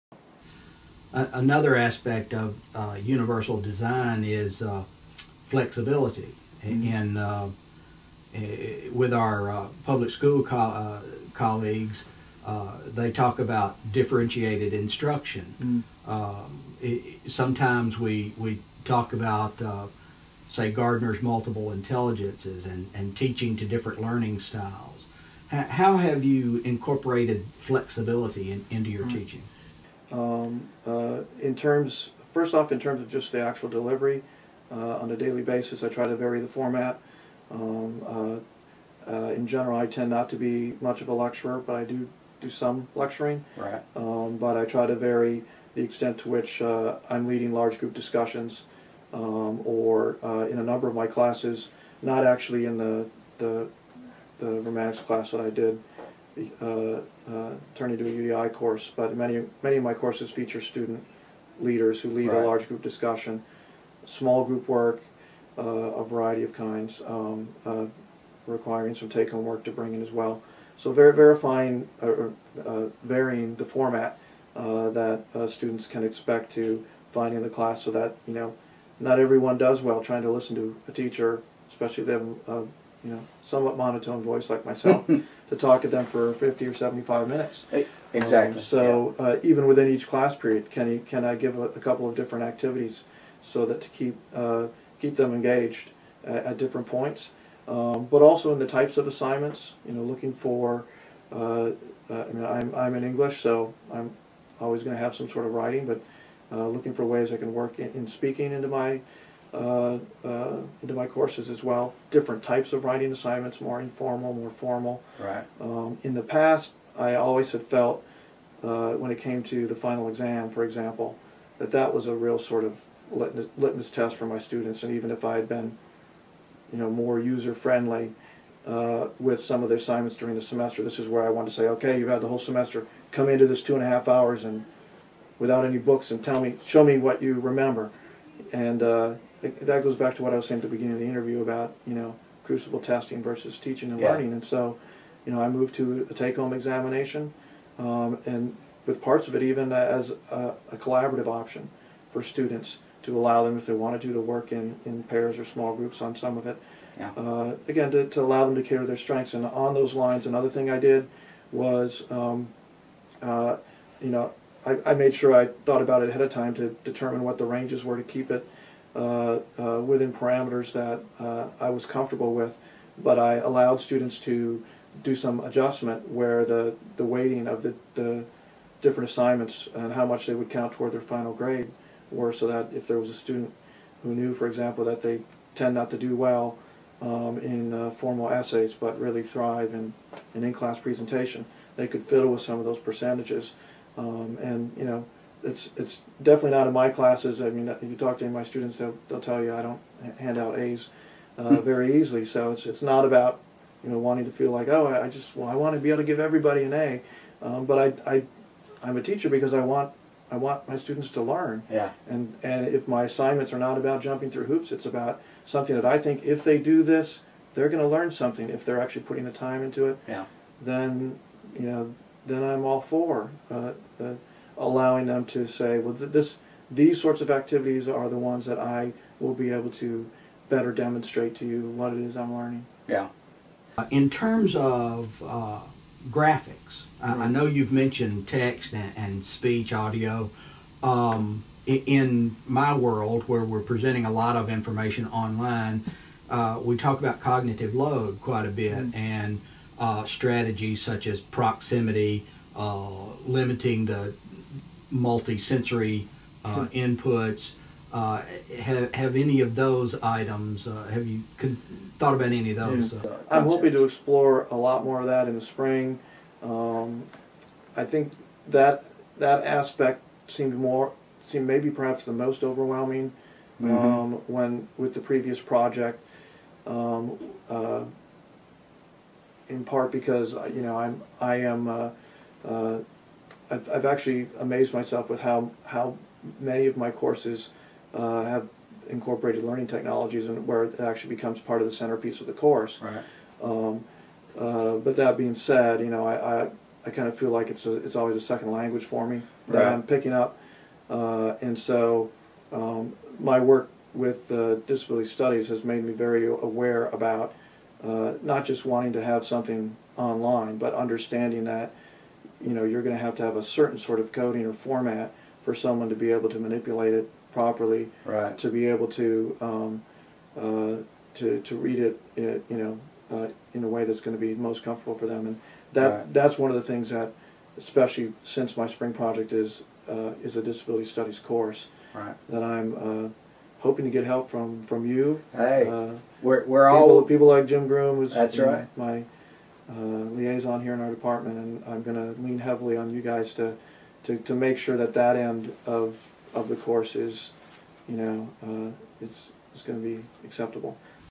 Office Chat: Universal Design for Instruction, Part 3 of 4
Dr. Chris Foss of the University of Mary Washington discusses the Universal Design for Instruction model and how he uses it with his literature classes.